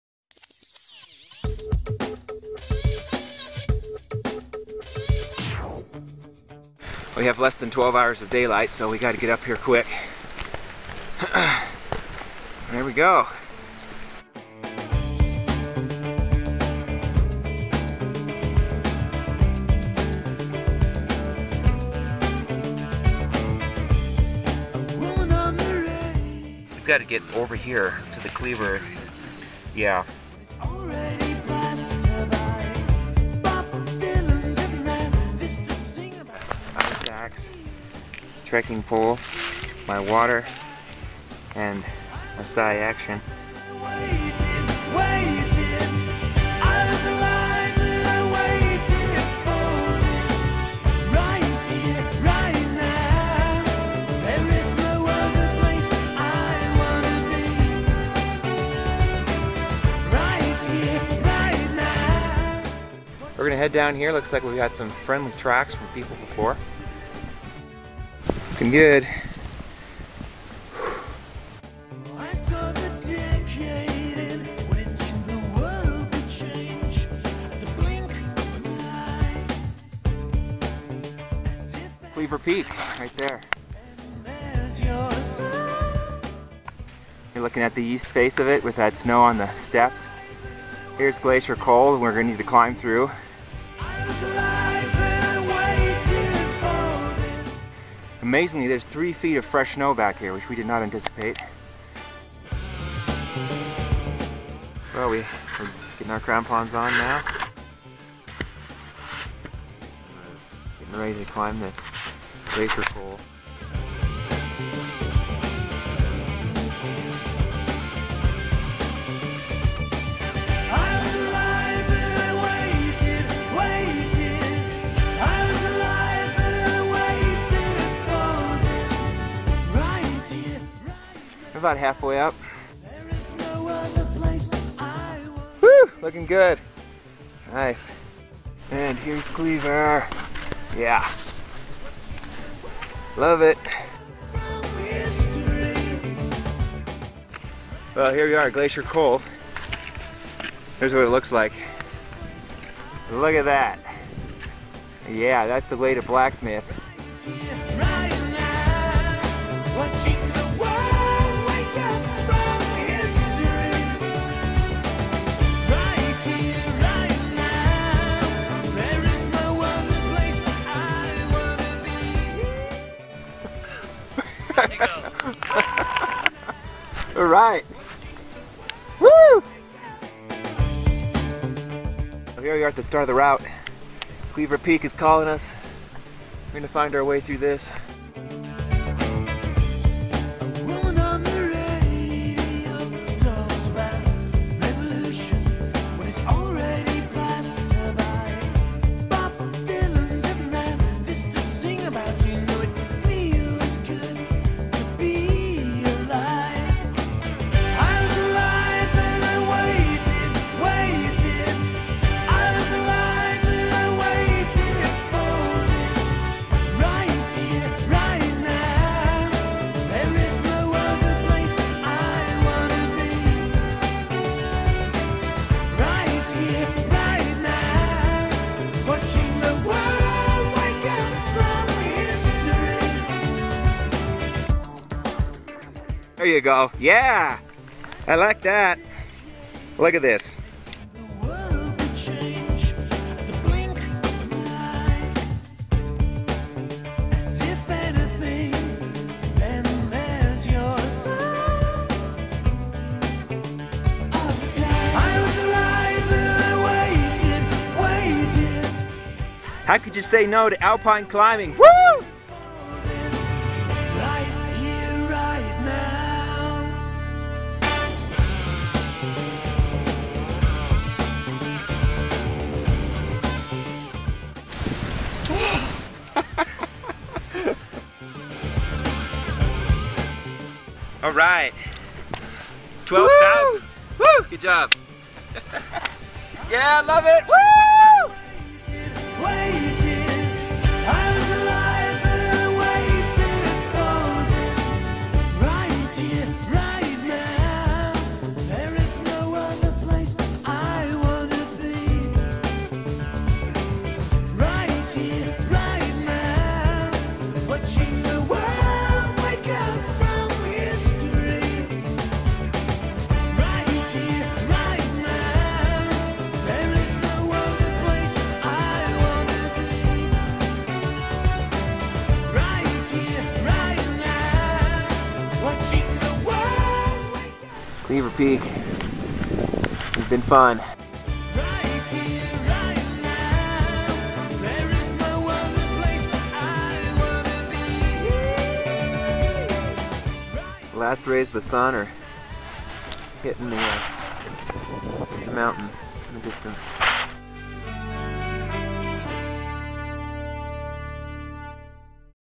Cleaver Peak - Sierra Mountaineering Club
Sierra Mountaineering Club on the seldom climbed and spectacular Cleaver Peak in the Sawtooth range, in one long 18-hour snow and rock climbing effort.